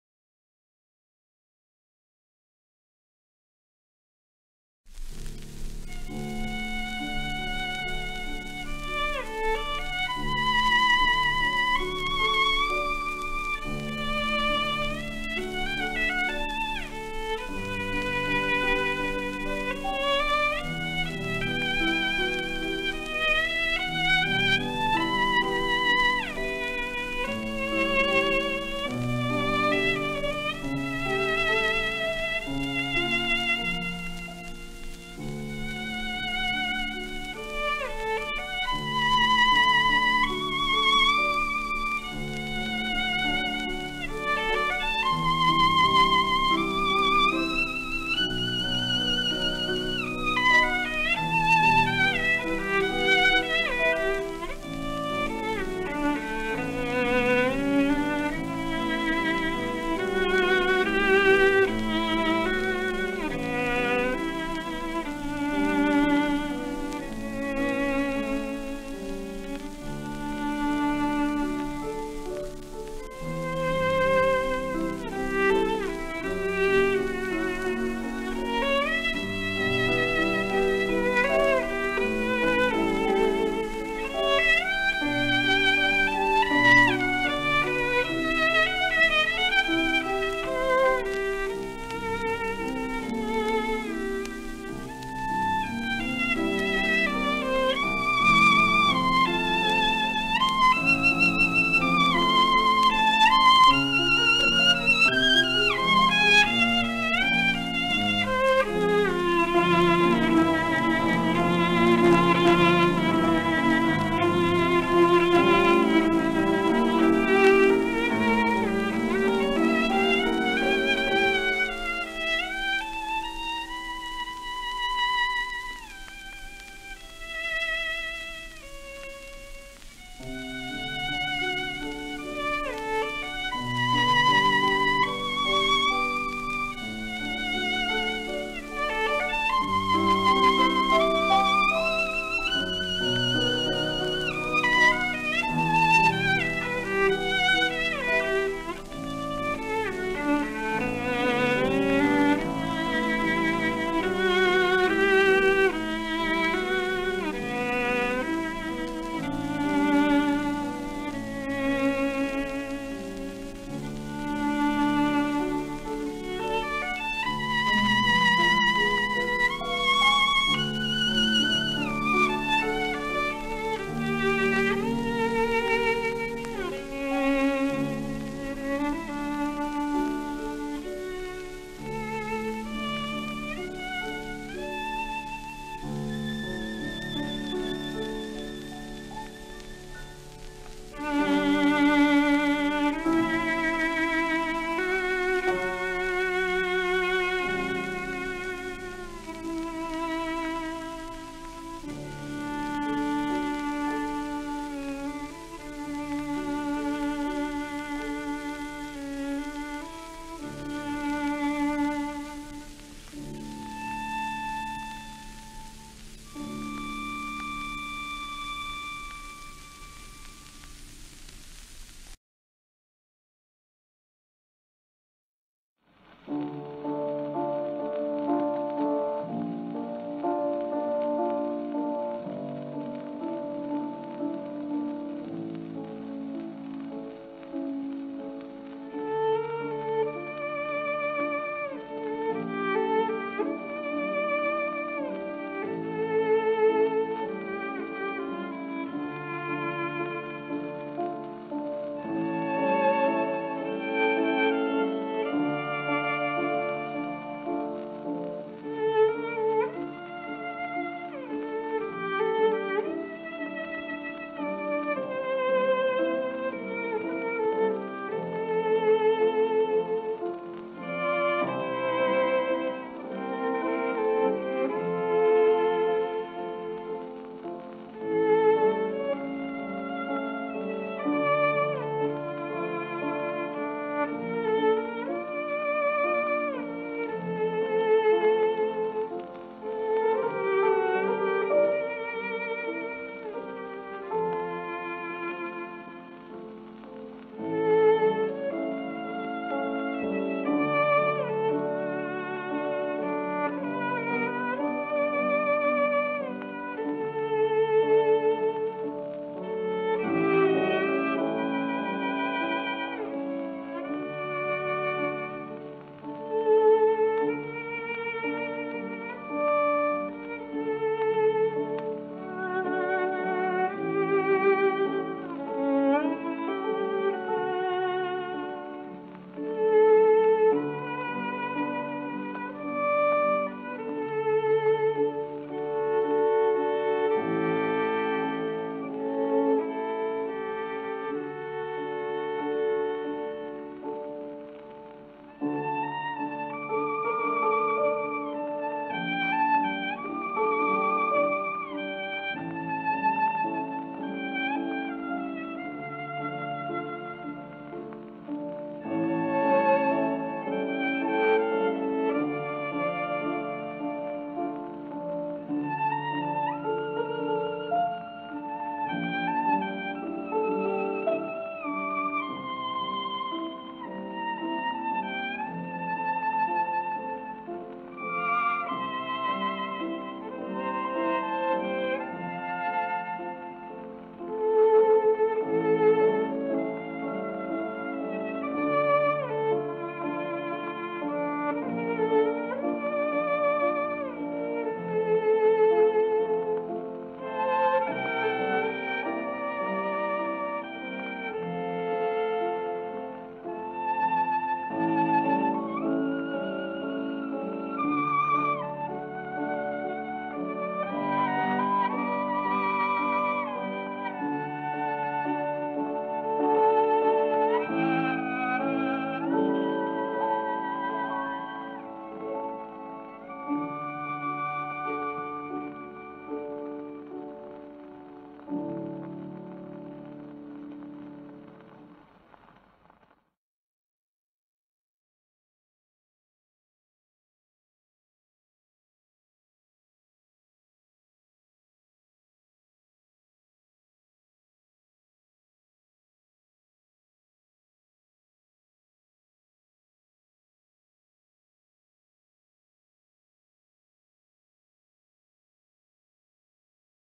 ナチス宣伝相ゲッベルスが、美貌の天才ヴァイオリニスト諏訪根自子（すわねじこ）に贈ったストラディバリウス Ver2.0
◆ 3年近く前に書いたブログ記事の要約＆朗報です。

　根自子さんの小説をフランス人作家Yoann Iacono氏が書いてくれてた（2021年出版）ことを知って感激し、衝動のままに初めて動画を作りました。後悔はしてません。
　どこかの出版社が日本語版を出してくないだろうか･･････|ω・`)ﾁﾗ
　しかし、1日使っただけでフォトの限界を感じました。あと何回か動画を作ったら有料の動画編集ソフトに挑戦してみるつもり。

・主な参考資料
諏訪根自子 美貌のヴァイオリニスト その劇的生涯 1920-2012